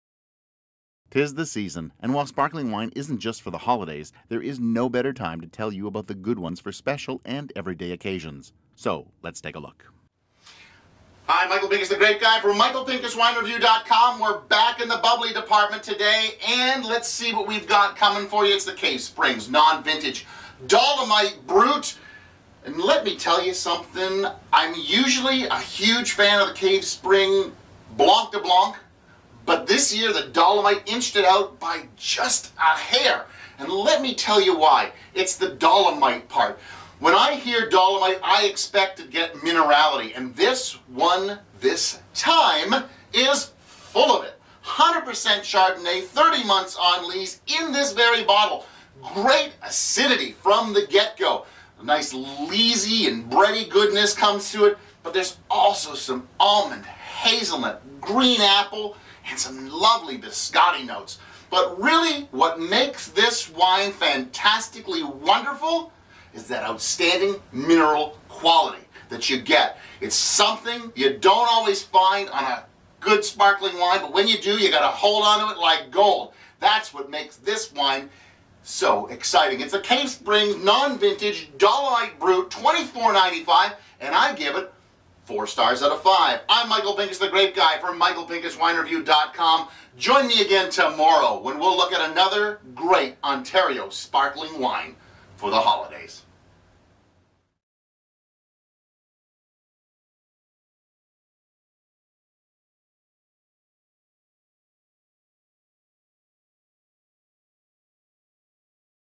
Sparkling Holidays Week: Cave Spring Cellars Dolomite Brut
Sparkling Wine is in the spotlight all week long as we celebrate the holidays all week.

Today's Bubbly is the Cave Spring Cellars Dolomite Brut ($24.95) available at the winery.

Join Michael every day this week as he reviews bottles of great Ontairo Sparklers.

And then join the Grape Guy every Wednesday as he reviews another bottle of interesting, exciting and always delicious Ontario wine.